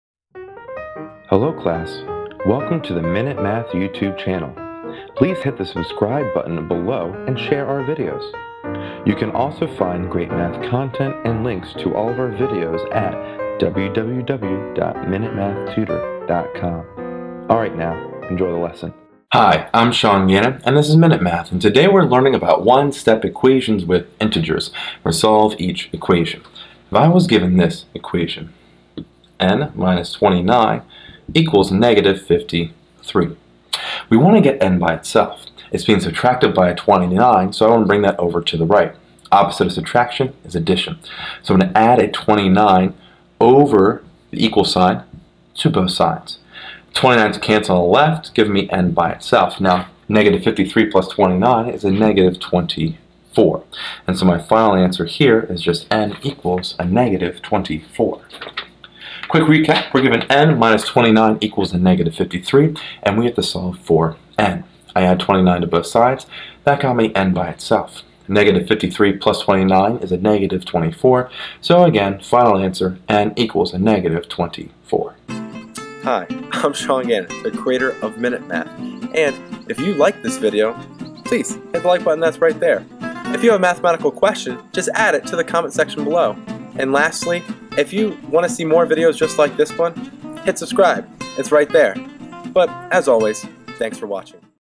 Solve n-29=-53
In this math video lesson on One-Step Equations with Integers, I solve n-29=-53.